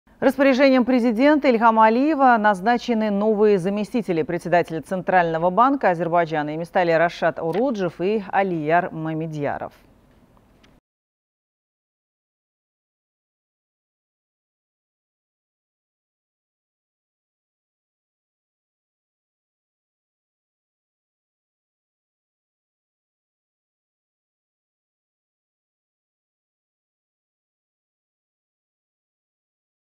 Назначены новые заместители председателя Центрального банка Азербайджана
Распоряжением президента Ильхама Алиева назначены новые заместители председателя Центрального банка Азербайджана. Ими стали Рашад Оруджев  и Алияр Мамедъяров.